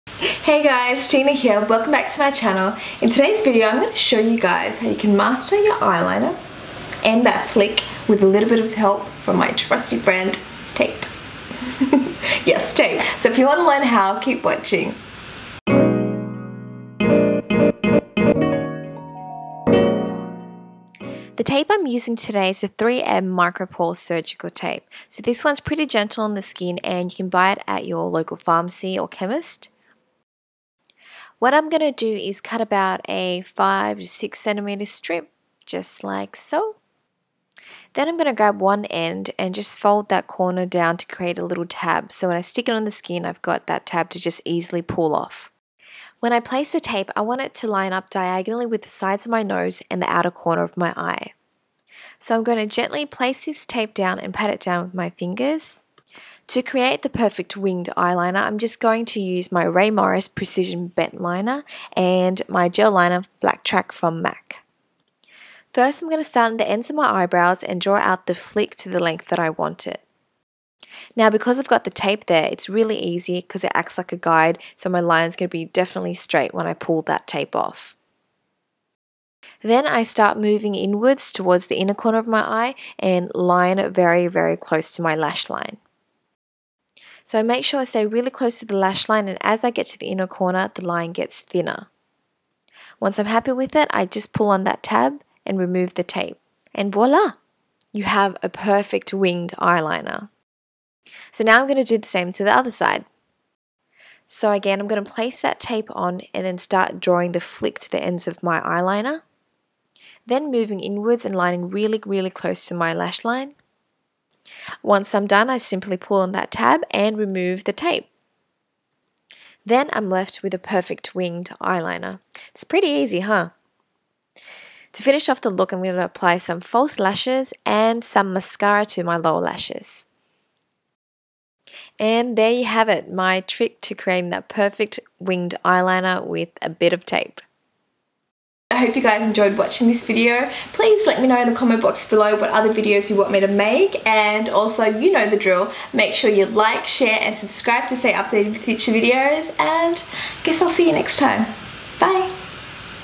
Perfect Winged Eyeliner Using Tape
Here's my secret to drawing the perfect winged eyeliner using tape!

Microphone: Rode Videomic Pro
Lighting: Ring Light
Camera: Canon 70D
Editing Program: Adobe Premier Pro